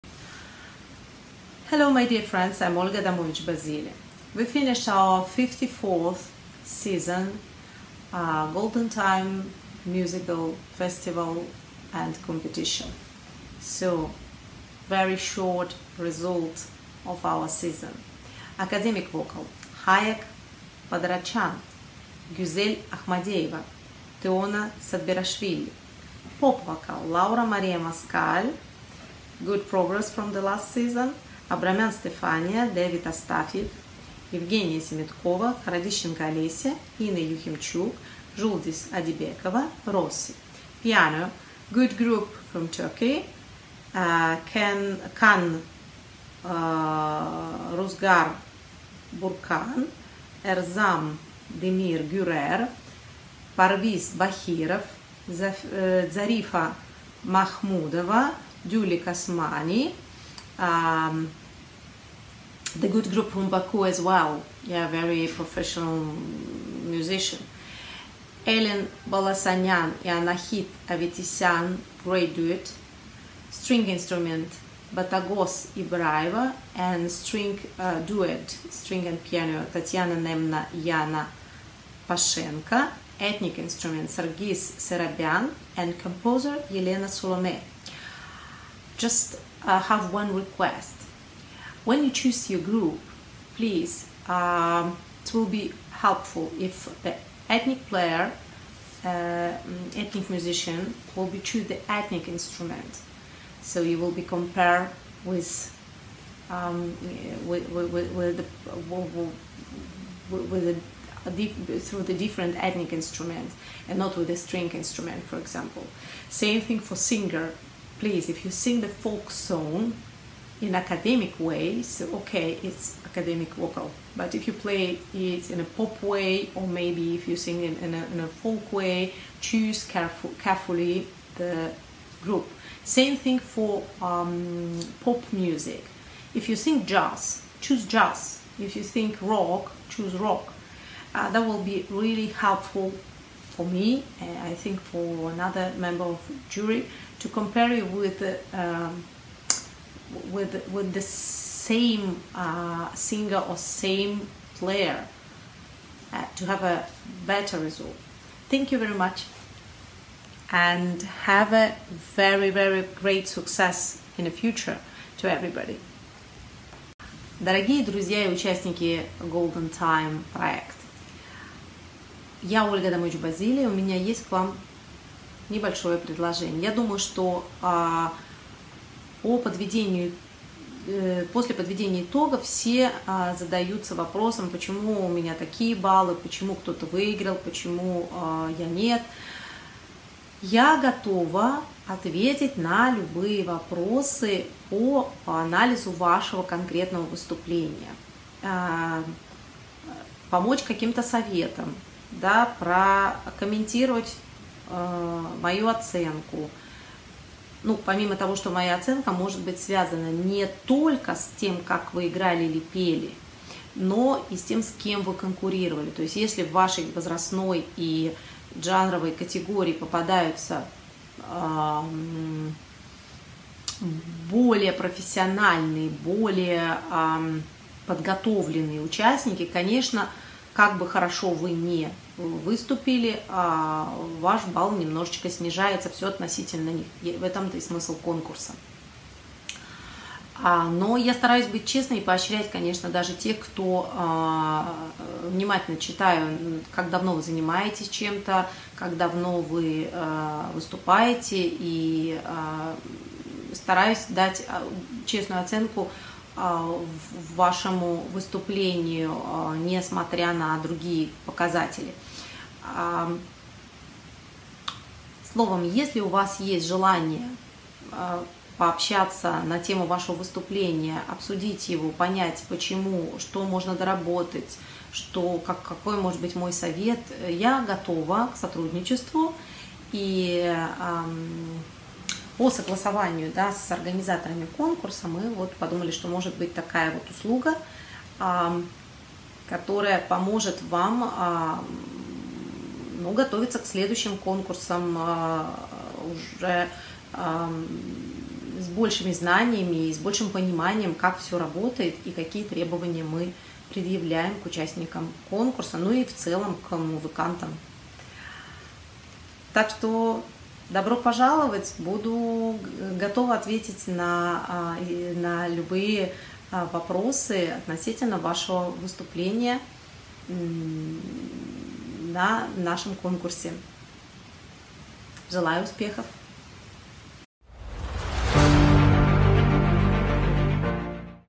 Видео оценка конкурсных работ 54 сезона - жюри Golden Time Talent - Olga Adamovich 🇬🇧
Дорогие друзья!
Публикуем видео оценку работ 54 сезона от жюри конкурса Golden Time Talent  - Olga Adamovich 🇬🇧  United Kingdom